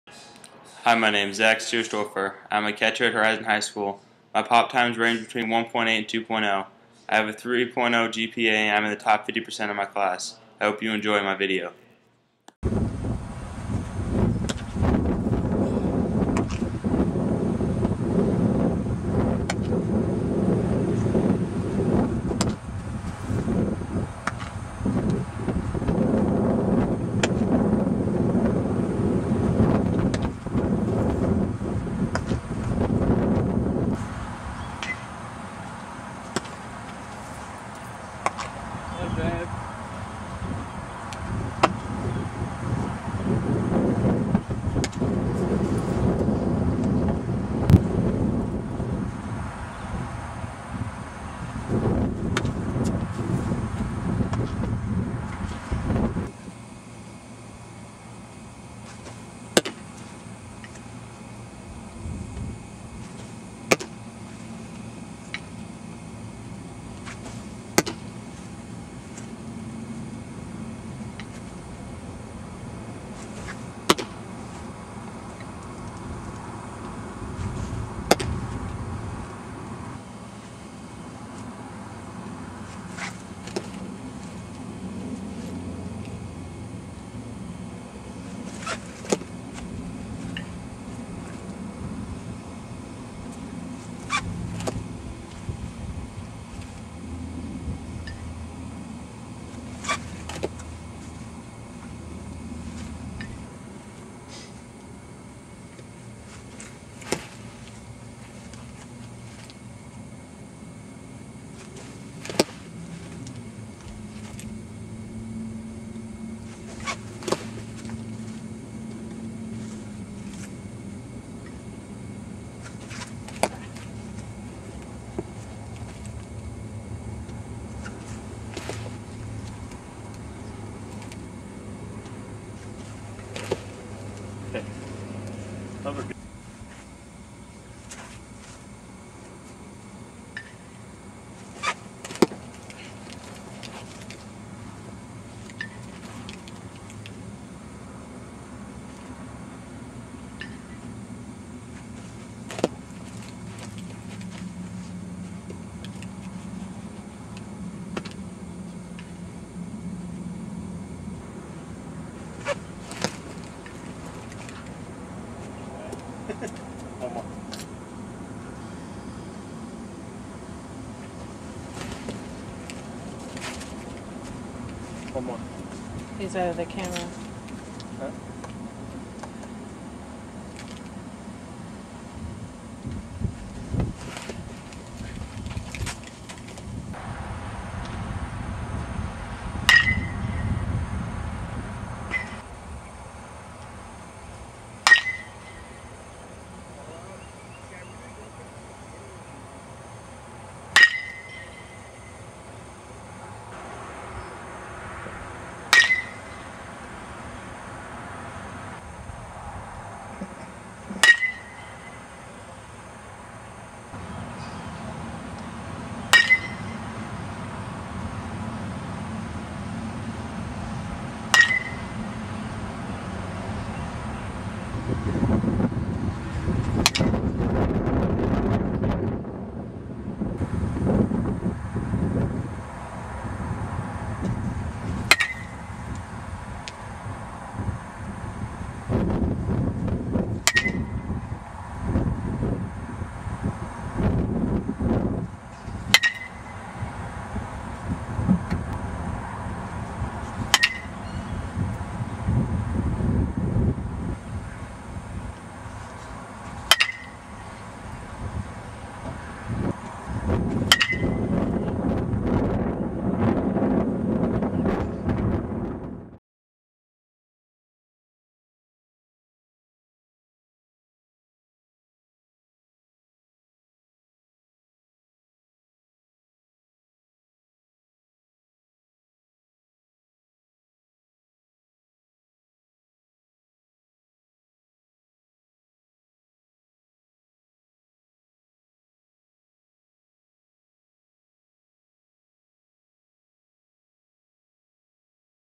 Zach Steirstorfer's Showcase
This is Zach Stierstorfer's college showcase video for college and professional viewing.